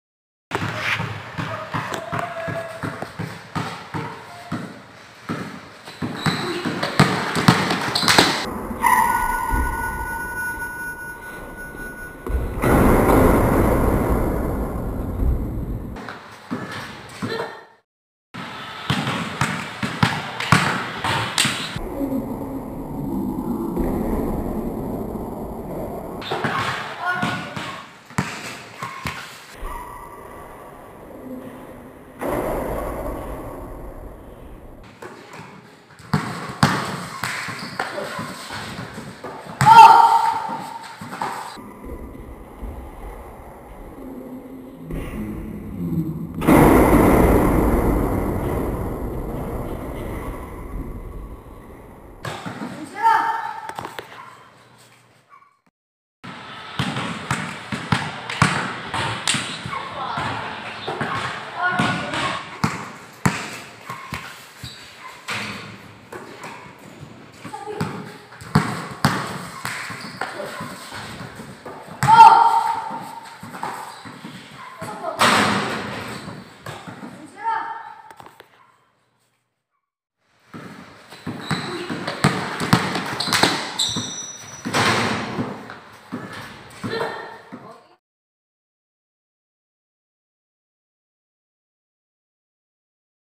BASKETBALL DEREK CYRUS CALDERON MALABAGUIO STYLE 111222SATURDAY AV1CAINTA
Friend of my 4 Grandchildren in our village in AV1 Cainta,Rizal.

Published: November 28,2022 Monday 12:13am
Category: Entertainment Sports Basketball
Filmed & Edited By Myself Vincent Victor at age 52